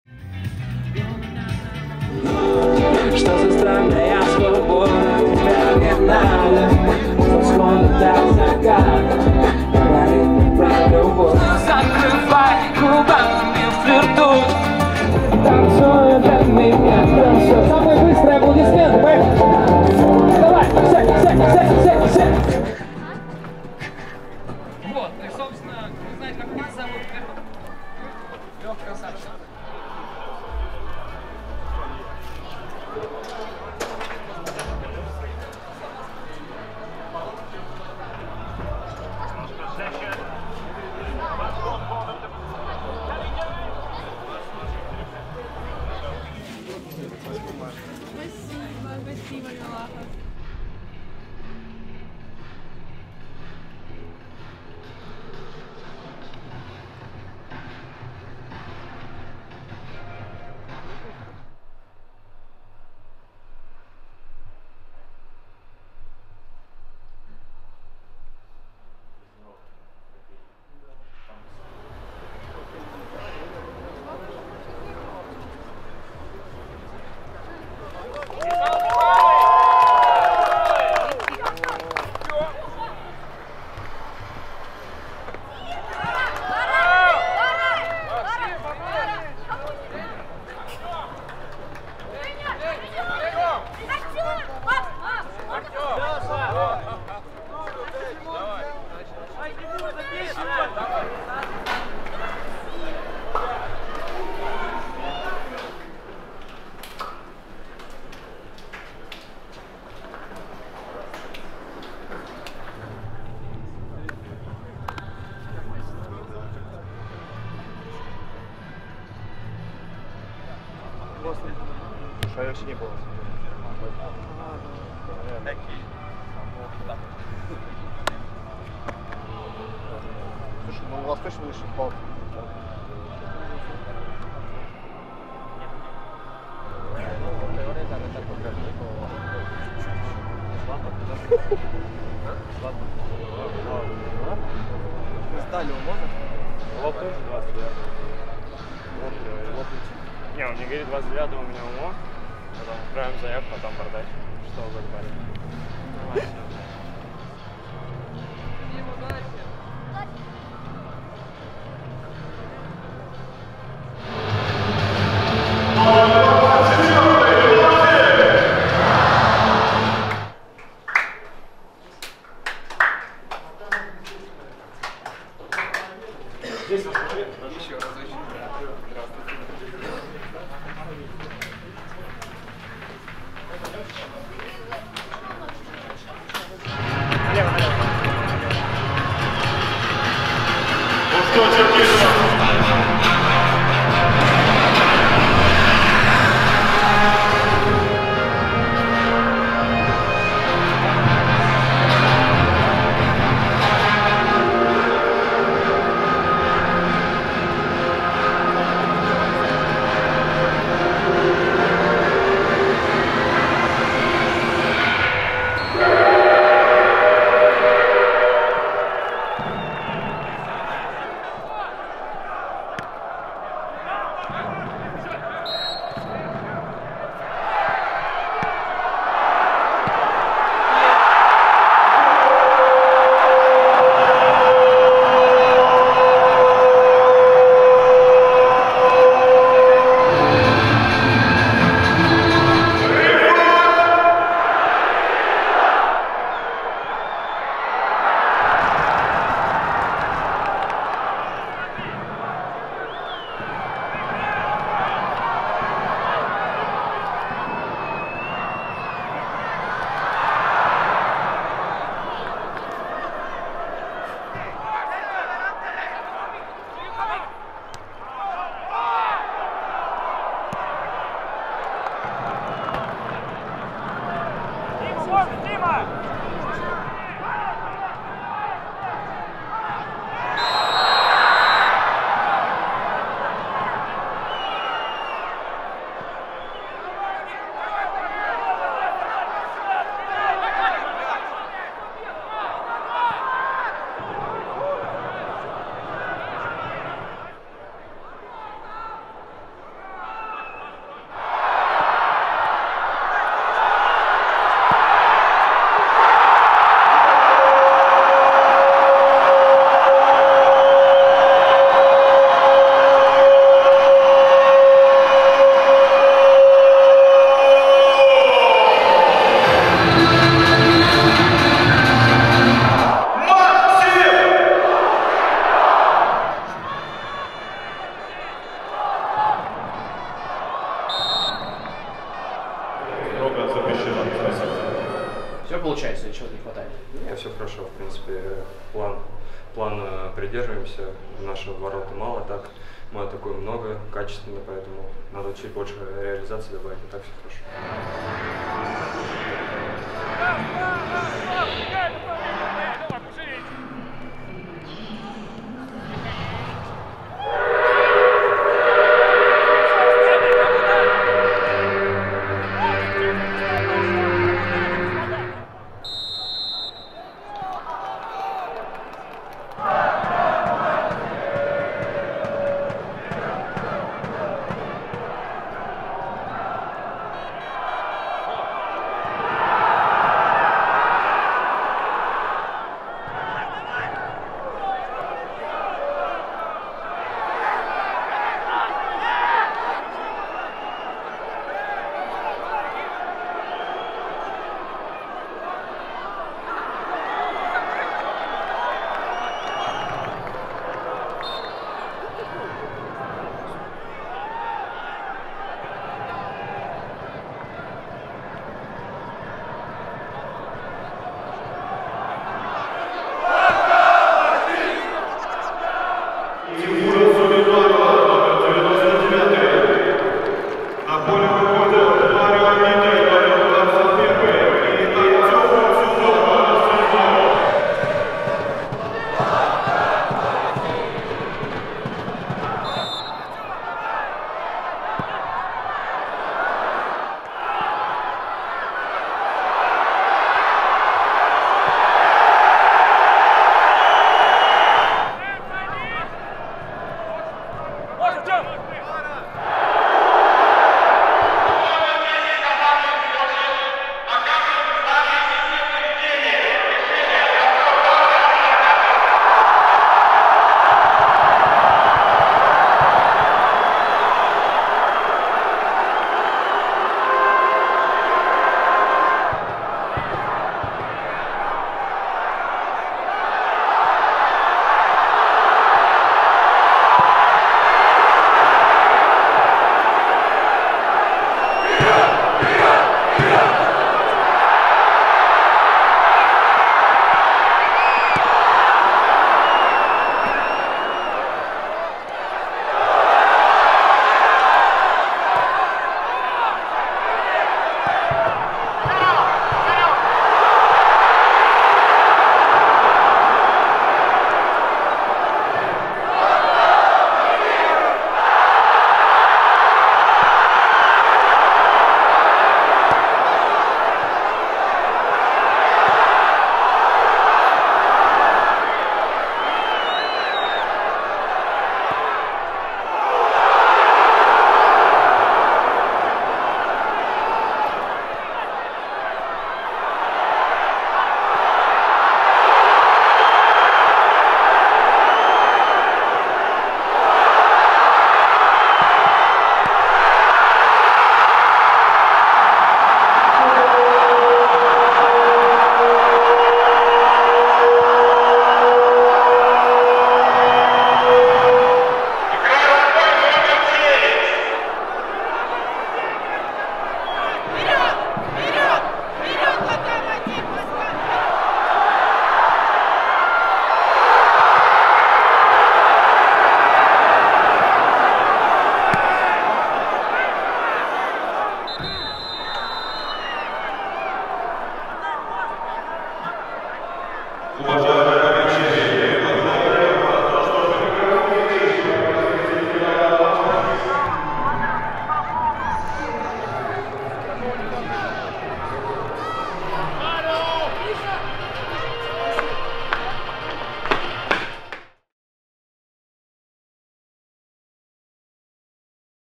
Loko Live | Напряжённый матч с «Сочи», голы Жемалетдинова и Глушенкова, убойная концовка
Блинов у нас нет, но есть новый выпуск Loko Live 🥞

Про голы, удаления и валидол мы уже говорили, а в этом ролике есть ещё много классного ⬇️

• Масштабный матчдей перед
• Подготовка стадиона к первой домашней игре
• Подслушанные разговорчики
• Ключевые моменты матча с другого ракурса
• Громкая поддержка болельщиков и эмоции команды после финального свистка

Включайте новый выпуск 😉